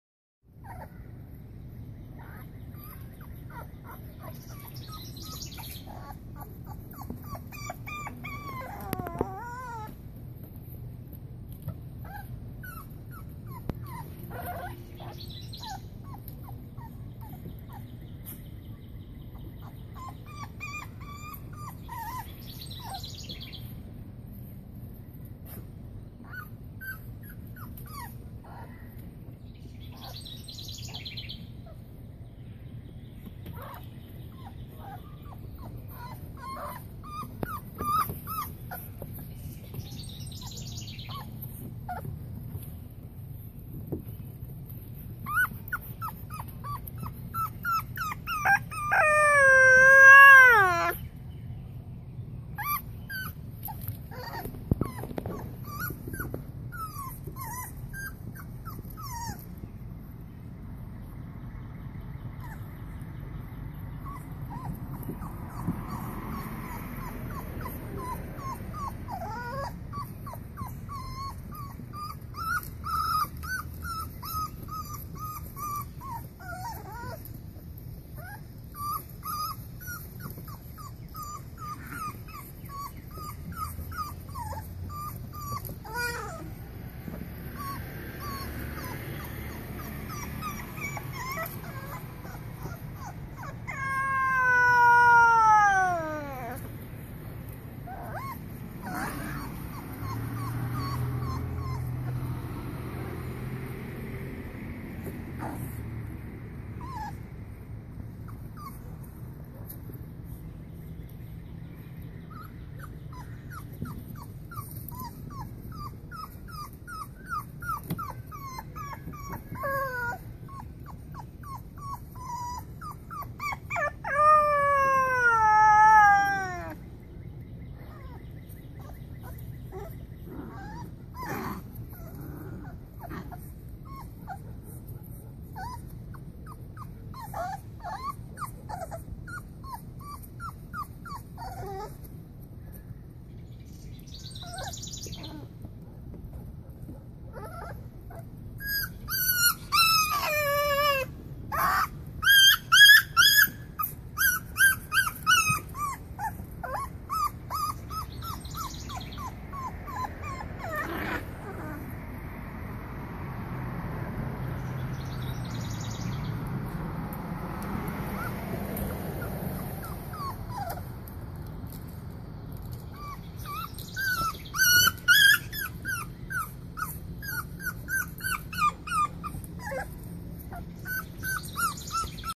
Boxers full of noise and play
Hudsons Halfway Home boxers getting some fresh air.